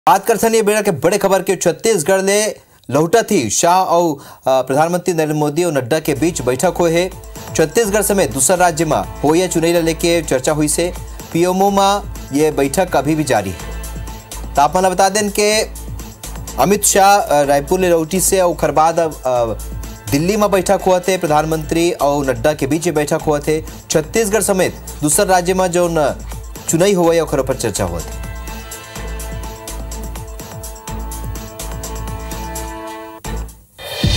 Chhattisgarh ले लहूटत ही शाह, PM अऊ नड्डा के बइठक। राज्य म होवइया चुनई ऊपर चर्चा
IBC24, Chhattisgarh ले लहूटत ही शाह, PM अऊ नड्डा के बइठक। राज्य म होवइया चुनई ऊपर चर्चा